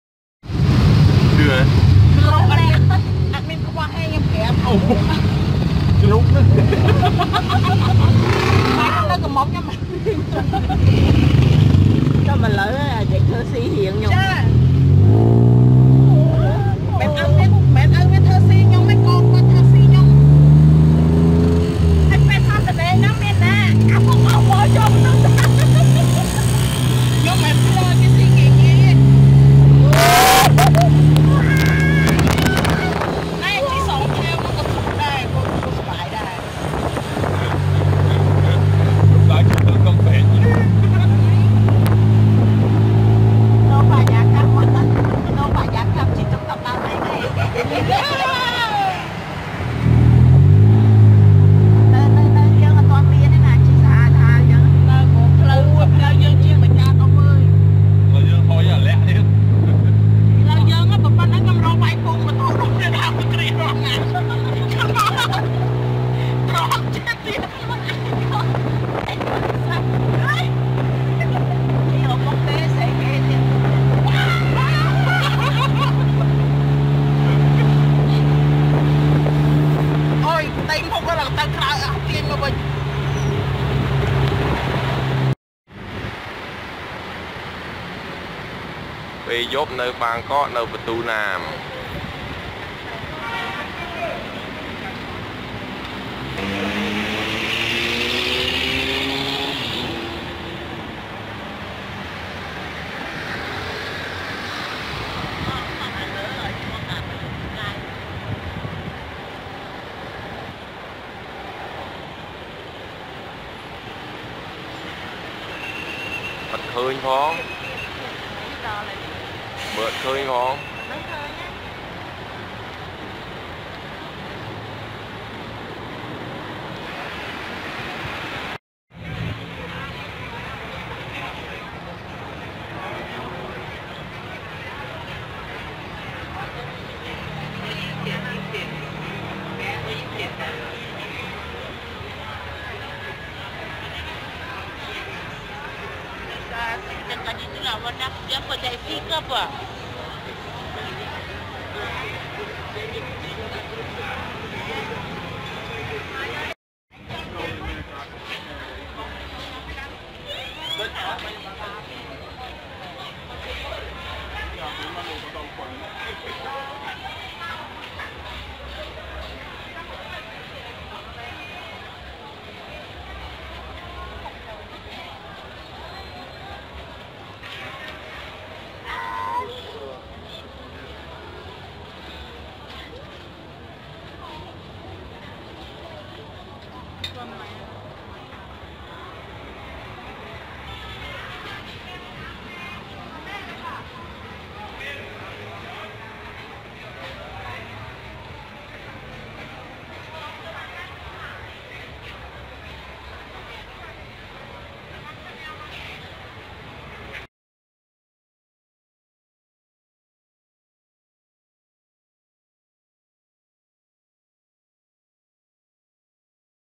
Pratunam on Street at Night
Visiting Pratunam night market,Bangkok-Thailand @PhengNaTalya